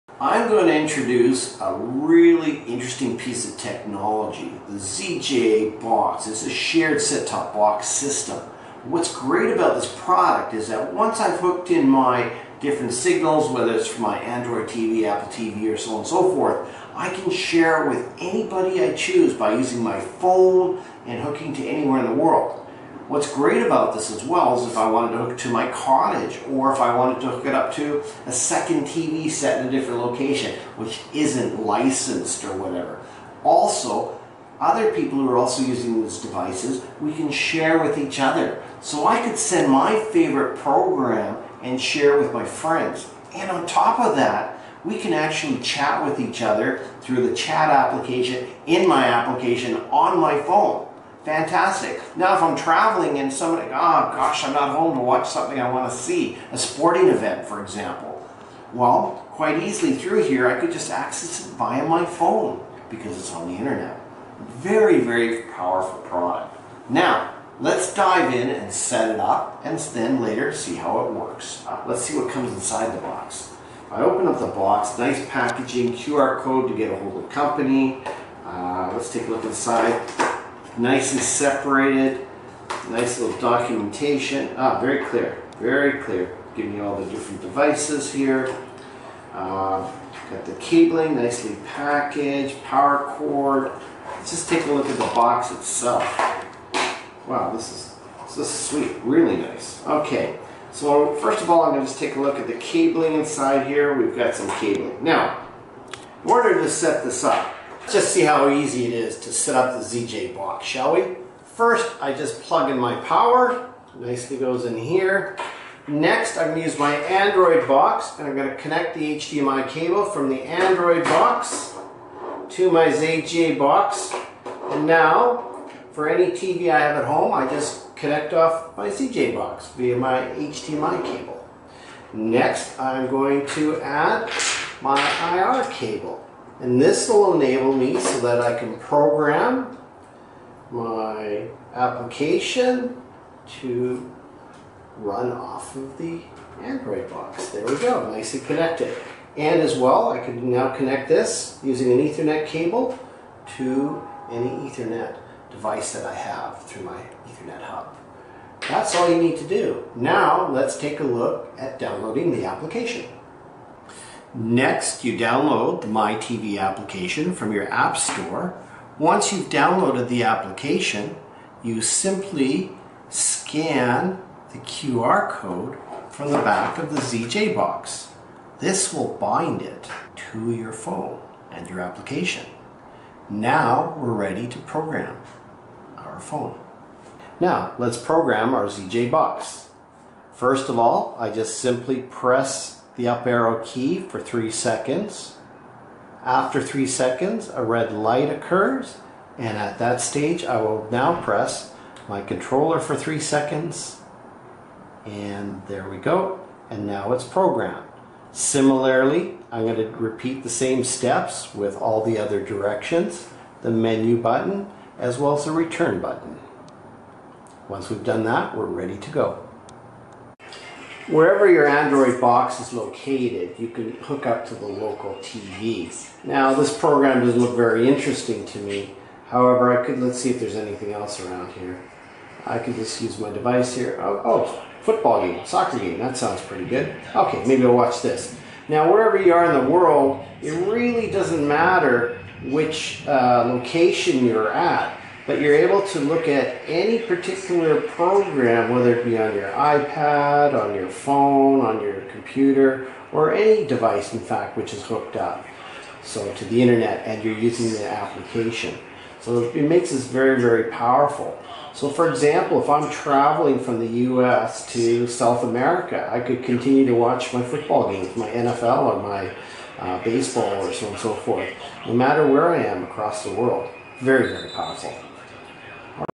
slingbox alternative,how to use and set up
here's the main features of the product:
1. watch home tv (set top tv box or android tv box) on mobile devices all over the world.
2. you can watch tv on 4 mobile devices(phone ipad imac et.) at the same time

3. you can watch other people's tv show if they agree to it
4. all you need to do is downloading the app,mobile tv anywhere